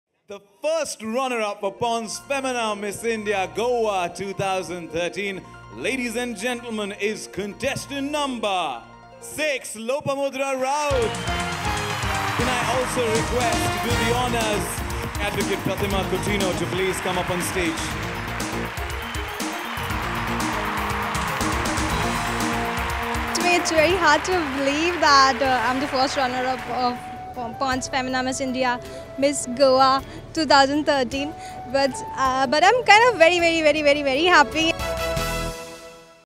Pond's Femina Miss India 2013 - First Runner Up - Goa: Lopamudra Raut
Name : Lopamudra Raut

Age : 22

City : Nagpur

Profession : Student, social worker.

Interests : Helping poor and needy, listening music, travelling, photography, partying, shopping and enjoying every part of life.

Crowned : Ponds Femina Miss India Goa '13: 1st runner-up, Enhance Femina Miss Body Beautiful, Sinhgad Femina Miss Intellectual

A quote or saying that has had a profound impact on you
If you want to fly with eagles, do not waste time in swimming with ducks!